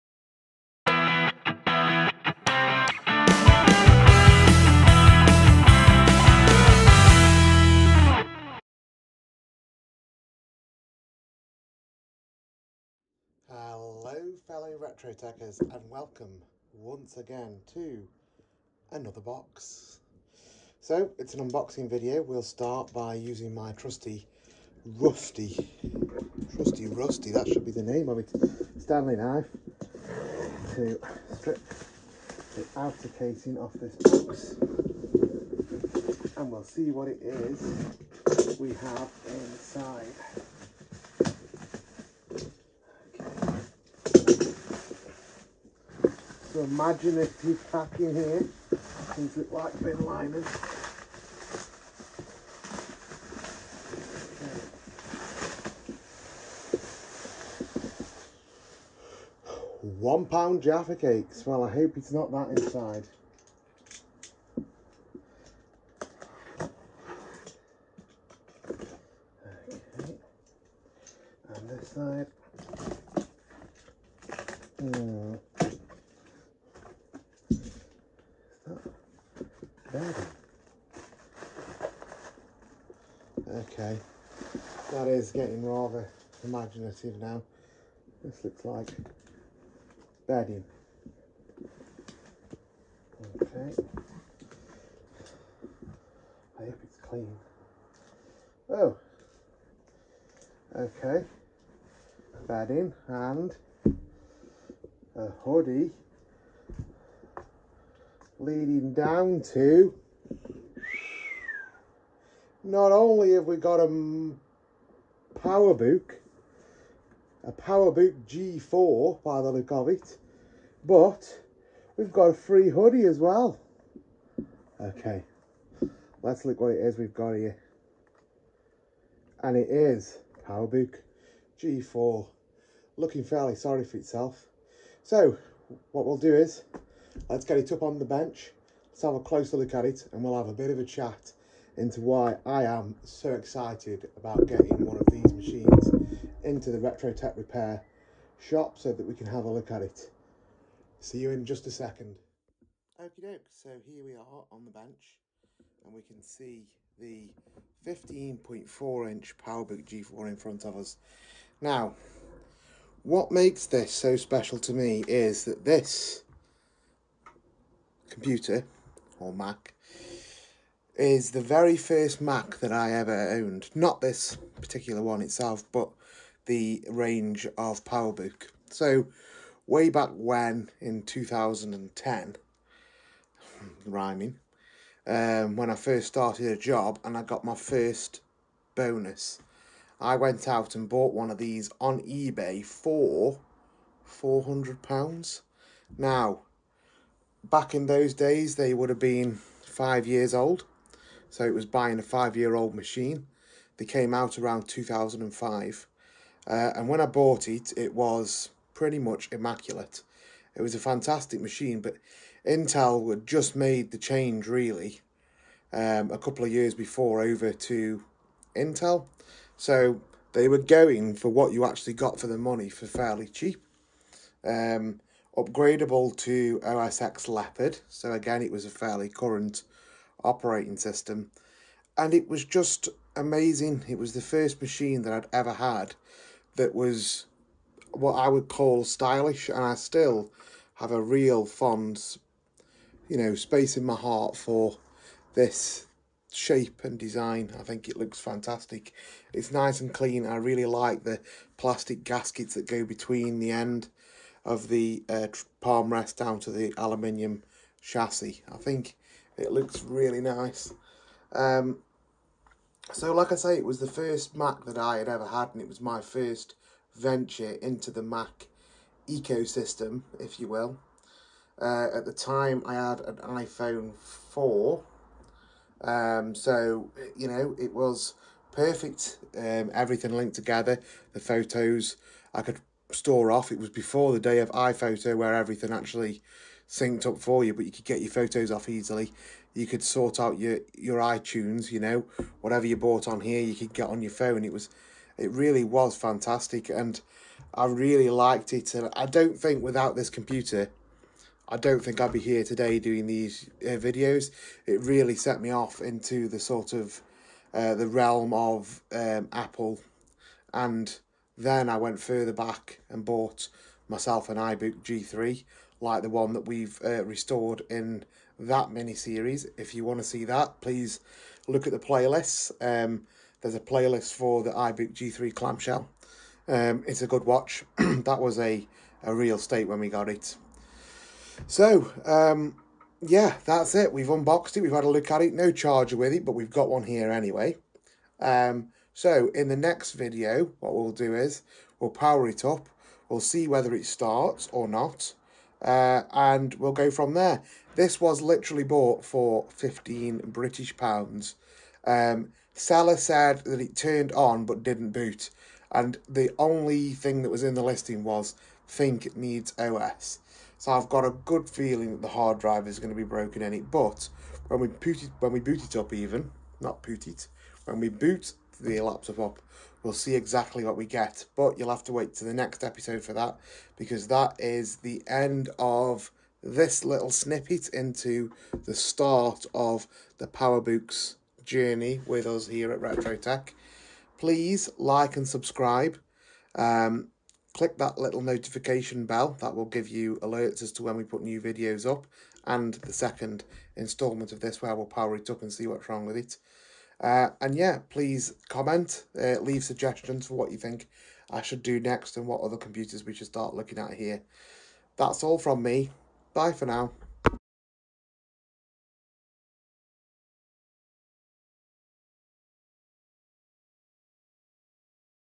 PowerBook G4 Episode 1 Unboxing
Episode 1 - PowerBook G4 Series

In this first episode of the new mini series from Retro-Tek Repair we unbox the legendary PowerBook G4!